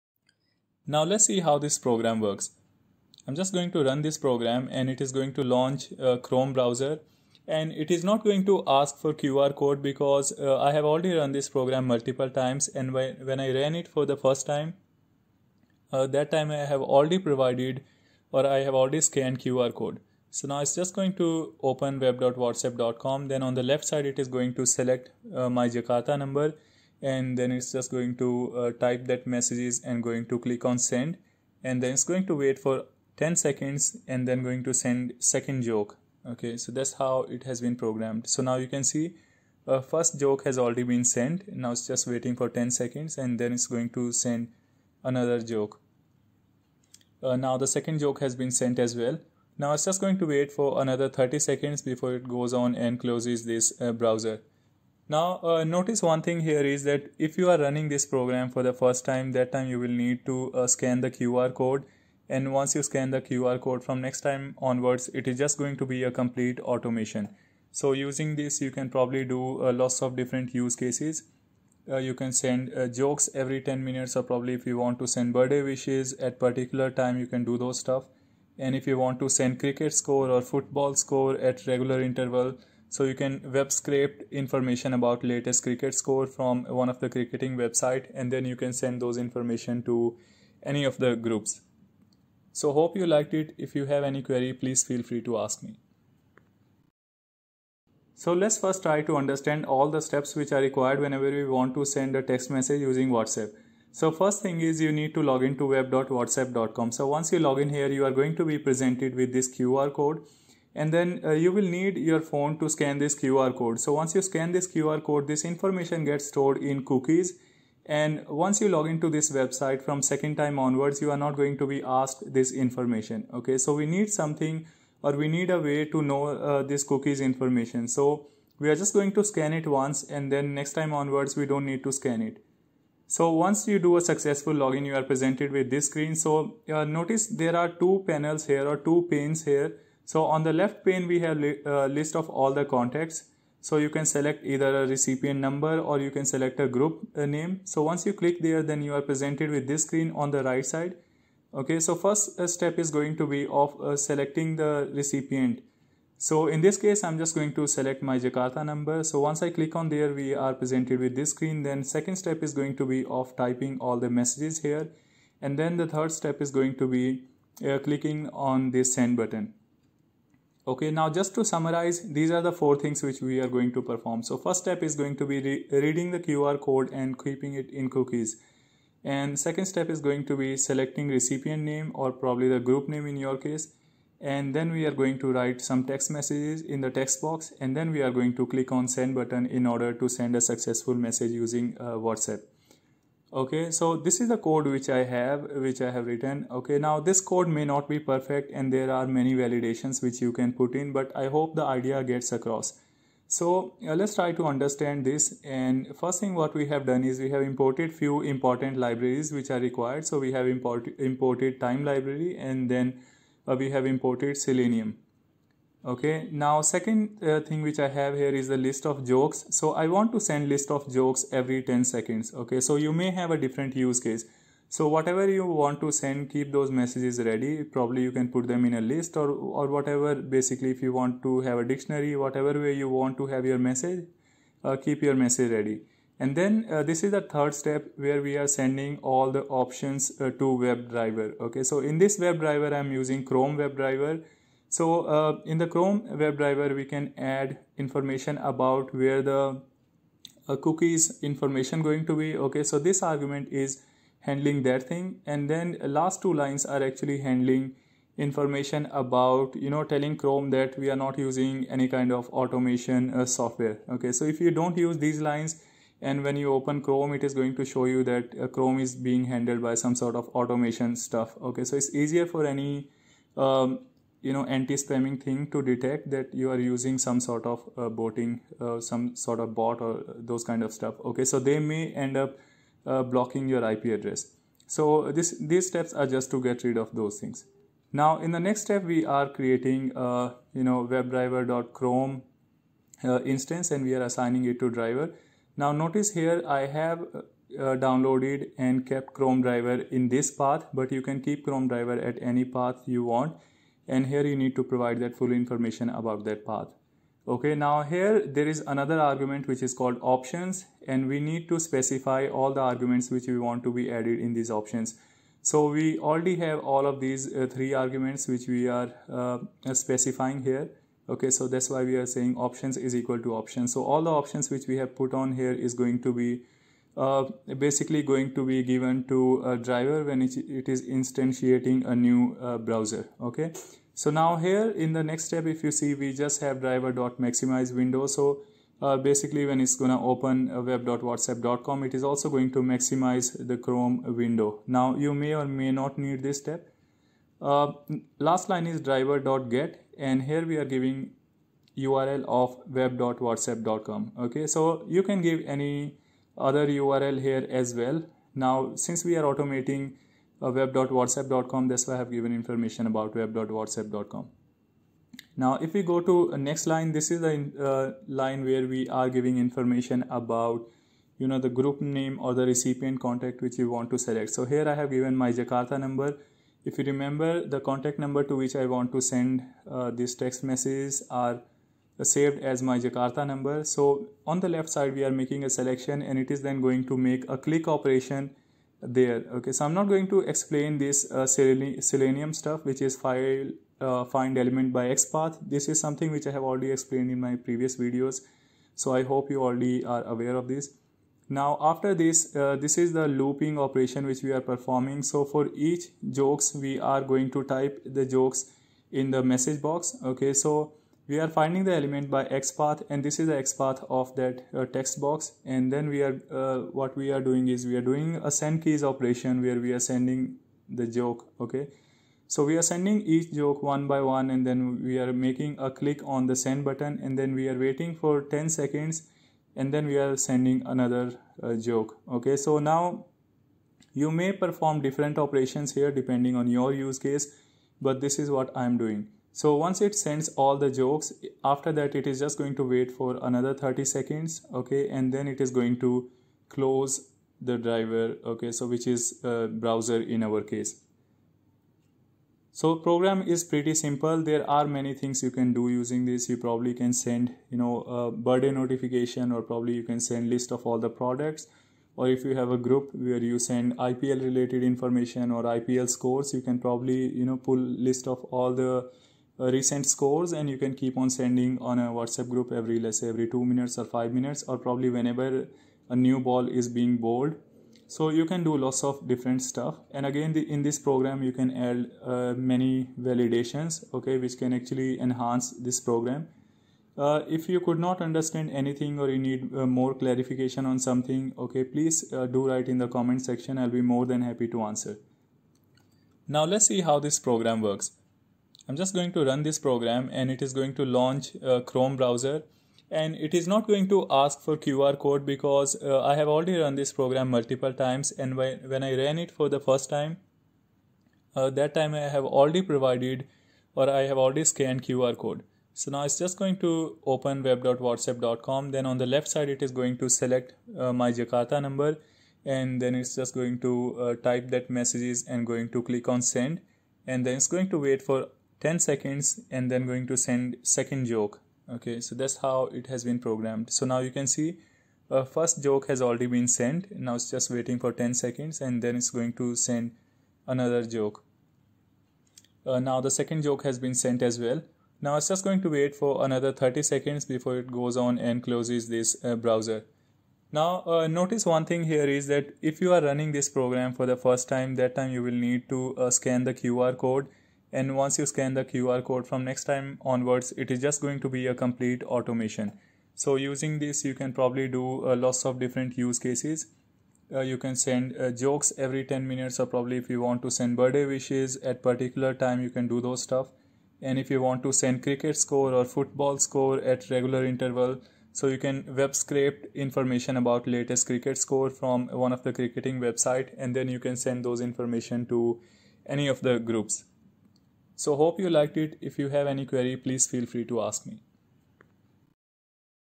Automatically send Whatsapp messages | Automate Whatsapp Messages Using Python
Want to connect on Instagram? Here is my id @vikasjha001 Connect to me:

In this video you are going to learn:
How to Automate Whatsapp messages
How to Automate Whatsapp Messages Using Python

This program does not have any dependency on any third party tool or Twilio. This Whatsapp automation is purely based on Python.

tep 1: Importing time and selenium Library. Time library will be used to perform sleep operations between operations. elenium Library will be used to perform Whatsapp Webscraping and web automation.

tep 2: We keep list of all jokes in a variable called “jokes”. Now this can be any list of messages which you want to send. This can also contain something dynamic such as latest product price, stock price, currency exchange rate etc. It just depnds on what is the use case you have.

tep 3: In this step we are telling chrome about cookies location and some other options which are important. I don’t want to complicate by discussing this in more detail.

tep 4: Here we are pointing to chrome driver. Please note that you will need to download chromedriver.exe which is suitable with the version of chrome you have installed on your computer. We are also telling the URL of web.whatsapp.com here. “driver.get” operation is going to launch a chrome browser session and going to open web.whatsapp.com for us. At this point of time you will need to scan the QR code if you are running this program for the first time.

tep 5: We are performing the steps of identifying contacts element and clicking on the the recipient name. Please note in this case recipient name is “MyJakartaNumber”. You can replace this with any contact name or group name you want to send message to.

tep 6: In this step, we are performing a loop operations to loop through all the jokes we have. Each joke is going to be written in the text box and send operation is going to be performed. Please note that I have put a sleep of 10 seconds in the loop. It means that from one joke to another there is going to be a delay of ten seconds.

tep 7: In this step we are just giving 30 seconds of sleep before closing chrome session.
Hopefully you liked the program and understood it. If you have any queries or suggestions, feel free to write to me.
You can also watch the video on my Youtube Channel to understand the whole process in more detail and see the program executing.

✈️ Travel Channel
ign up to killshare using this link and get one month free membership.